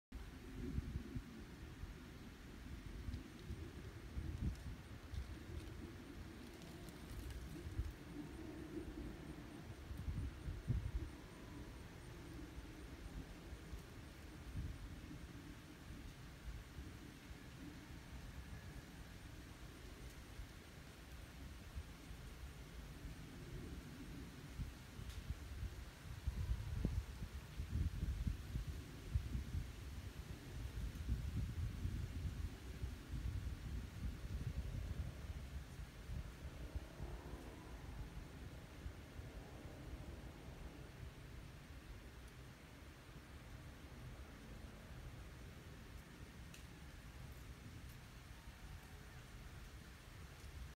United Airlines Boeing 757-200 [N19117] takeoff from MCO Airport from my house
United Airlines Boeing 757-224 [N19117] takeoff from Orlando International Airport (MCO) landing in New Newark Liberty International Airport (EWR)

Flight Info
Airline: United Airlines
Aircraft: Boeing 757-224
Reg: N19117
Livery: Normal
Airport: Orlando International Airport
Runway: N/A
Flight: UA2384
Route: MCO-EWR
Date: 11/22/18
Time: N/A